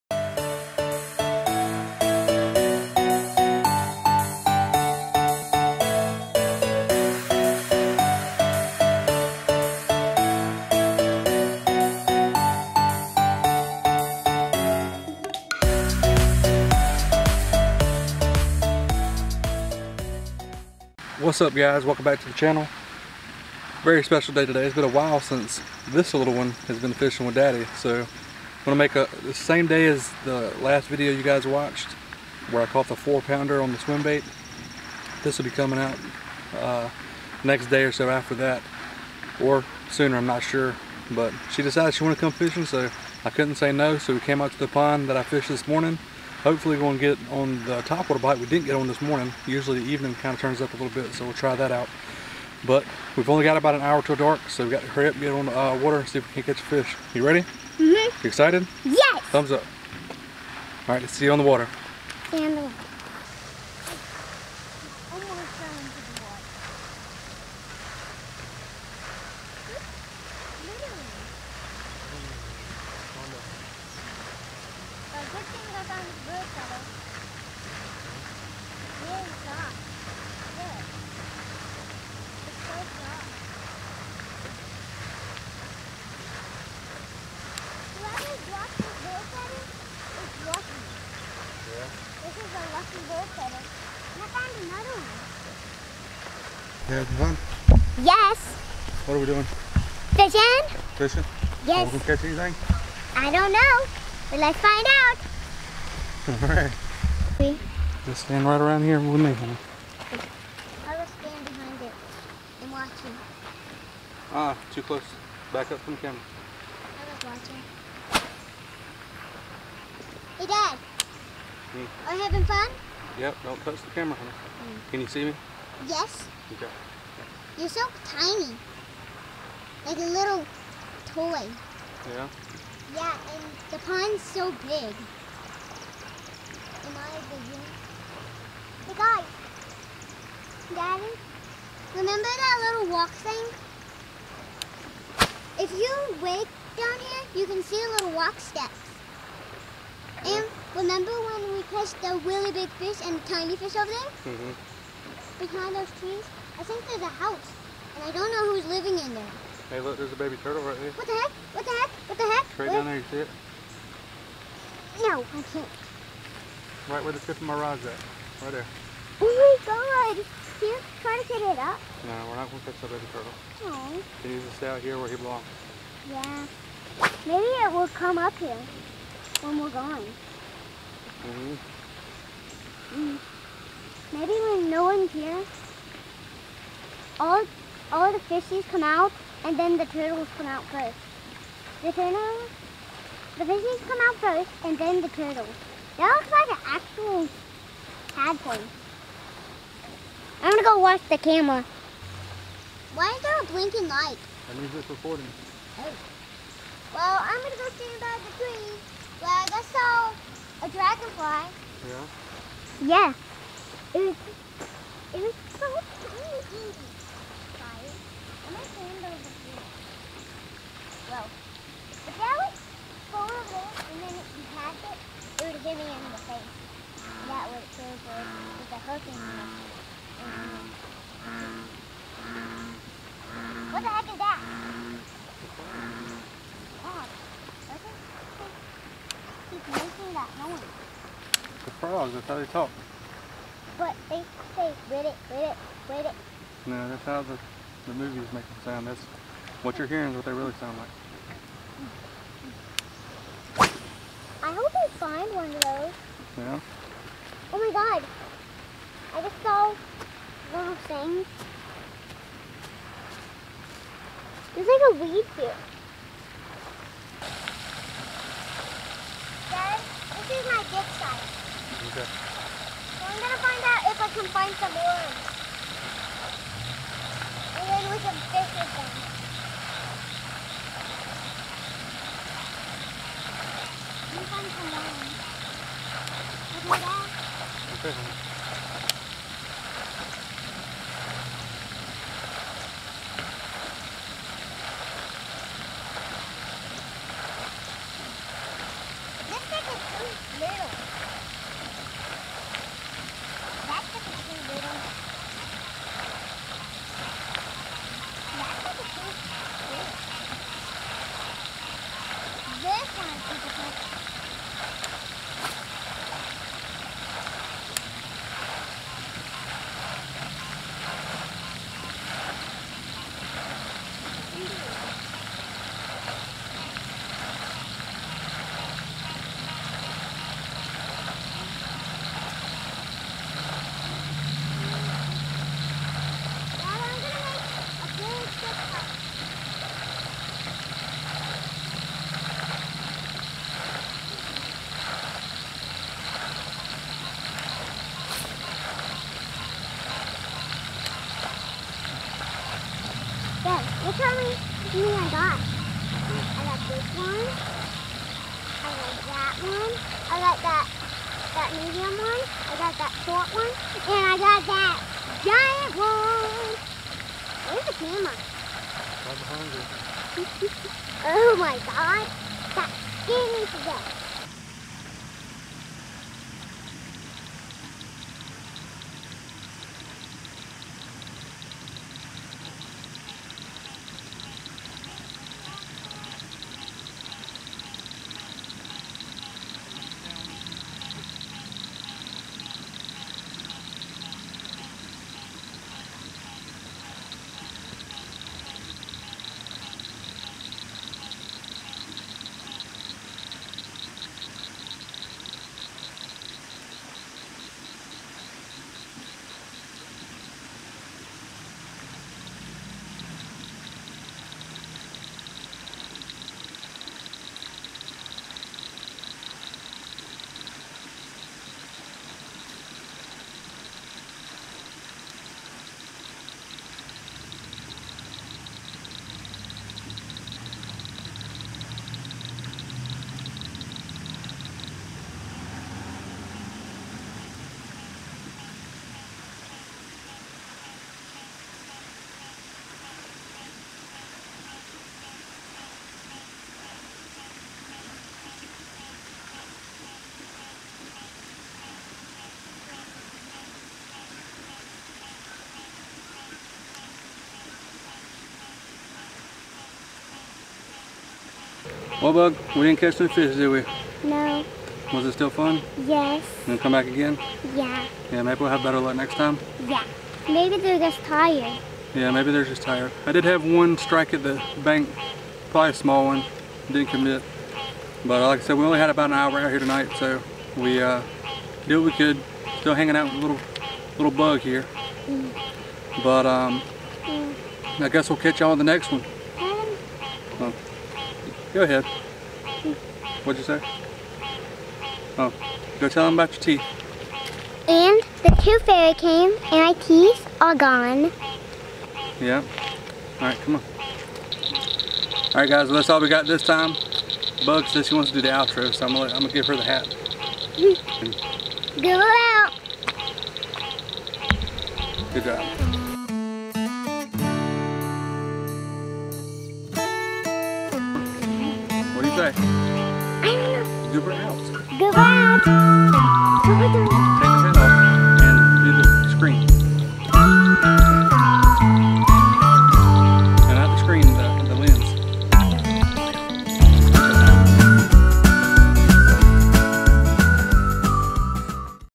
Daddy Daughter Fishing
Hey guys!
Guess who decided to come fishing with Daddy! Been a while but we had fun nonetheless!